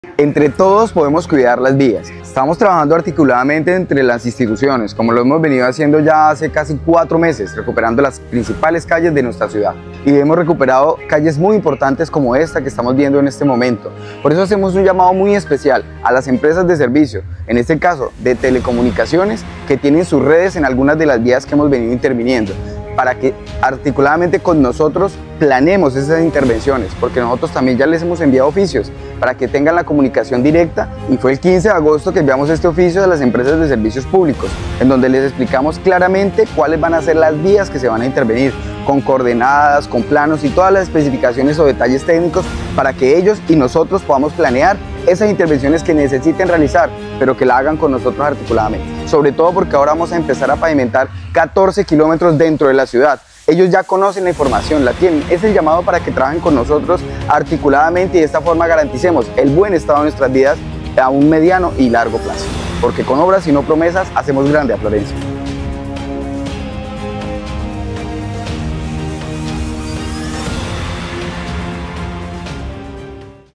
GOBERNACIÓN DE CAQUETÁ PIDE COOPERACIÓN A EMPRESAS DE COMUNICACIONES POR VÍAS.
La Gobernación de Caquetá solicita a las empresas de comunicaciones un trabajo en equipo por el bienestar de nuestras vías. Con el objetivo de mejorar la infraestructura vial y garantizar la calidad de las obras entregadas a nuestras comunidades, hemos solicitado que las empresas de telecomunicaciones planifiquen sus mantenimientos en las redes que pasan por las vías antes de que inicien las intervenciones viales.

Este esfuerzo conjunto asegurará que las mejoras viales sean duraderas y que el trabajo se haga con la máxima eficiencia para todos los caqueteños.